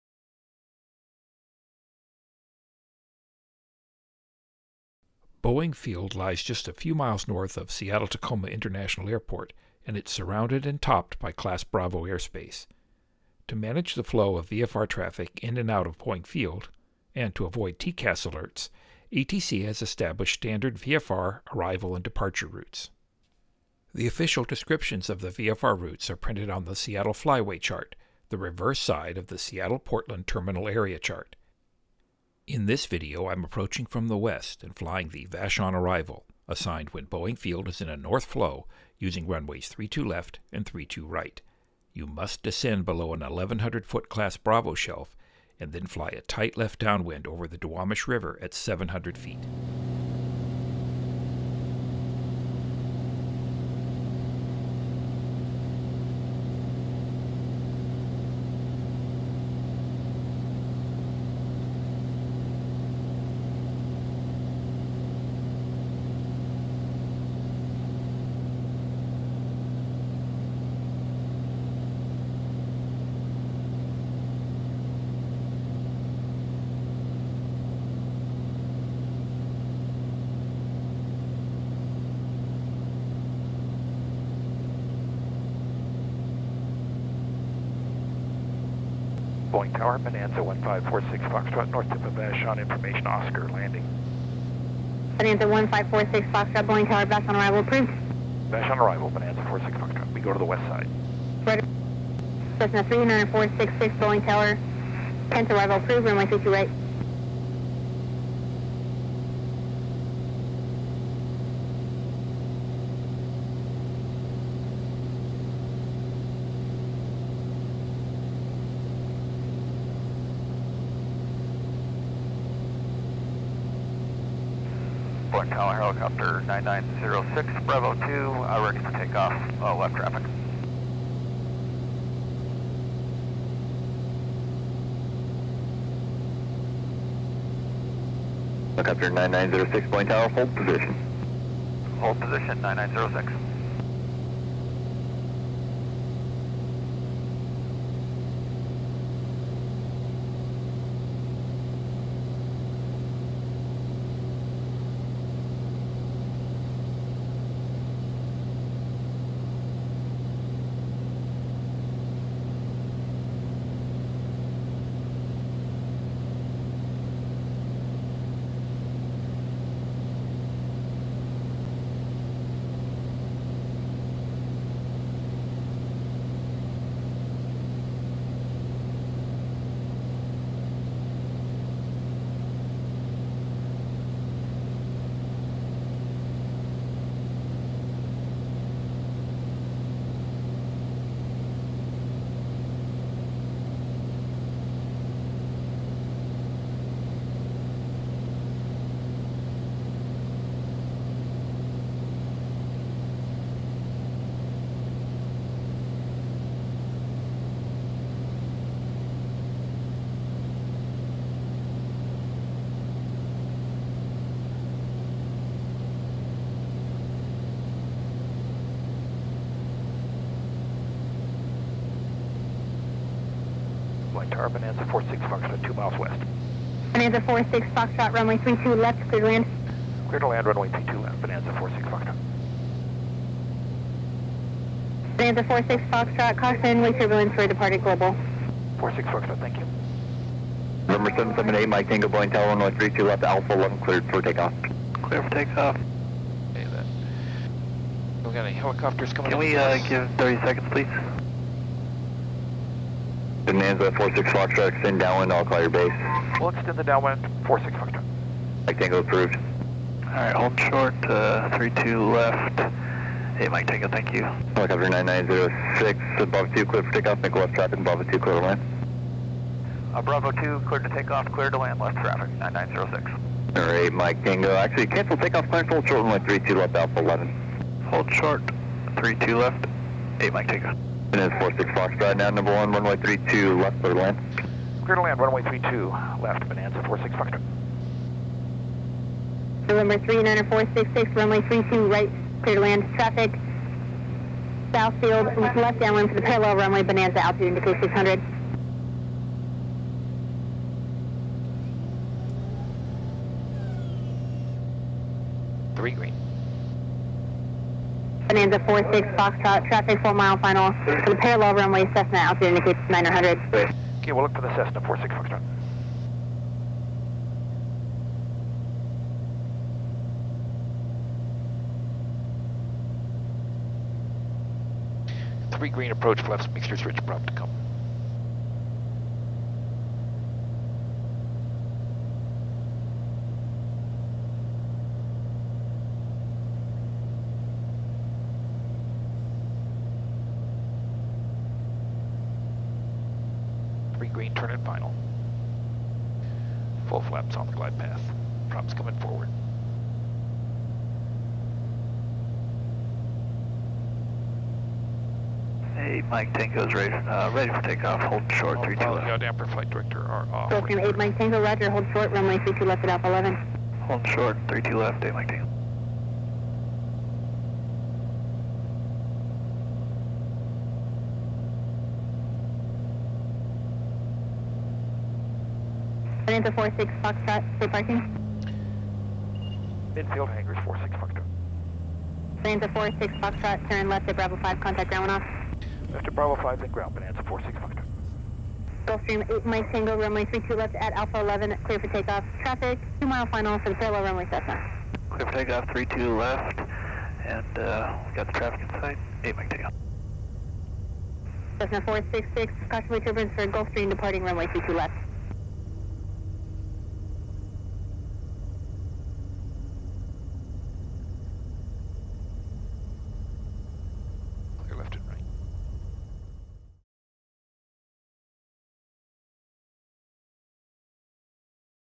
Flying the Vashon Arrival at KBFI
Boeing Field lies just a few miles north of Seattle-Tacoma International Airport, and it’s surrounded and topped by Class B airspace. To manage the flow of VFR traffic in and out KBFI and avoid TCAS alerts, ATC has established standard VFR arrival and departure routes.

The official descriptions of the VFR routes are printed on the Seattle Flyway chart, the reverse side of the Seattle/Portland Terminal Area Chart.

In this video, I’m approaching from the west and flying the Vashon Arrival, assigned when Boeing Field is in a north flow, using runways 32L and 32R. You must descend below an 1100 ft Class B shelf and then fly a tight left downwind over the Duwamish River at 700 ft.